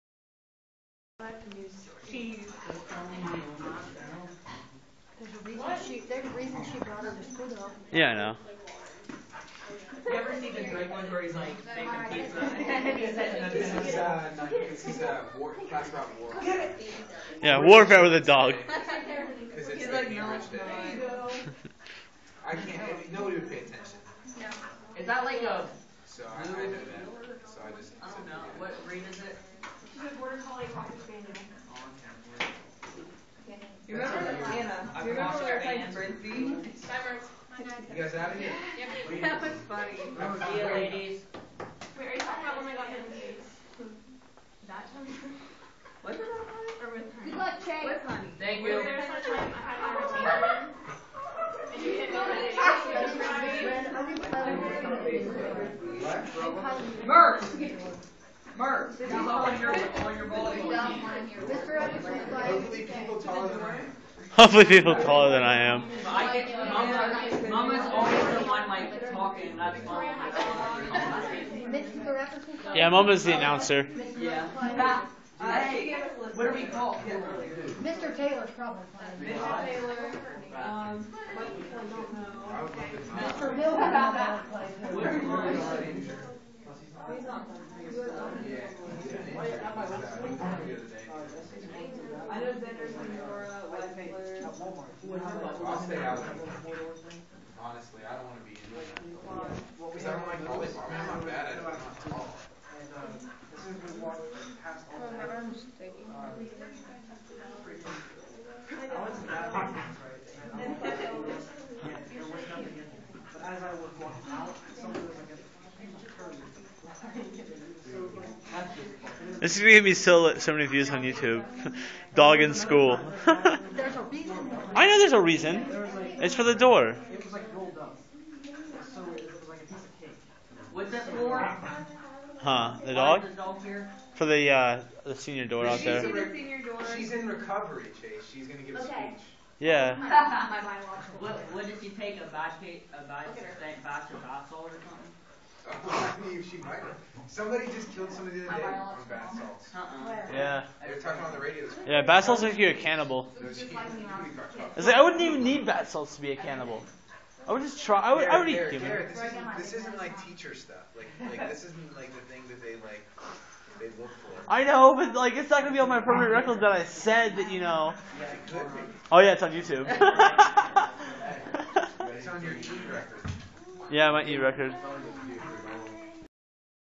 Girl brings dog to class
Just playing catch with a dog in school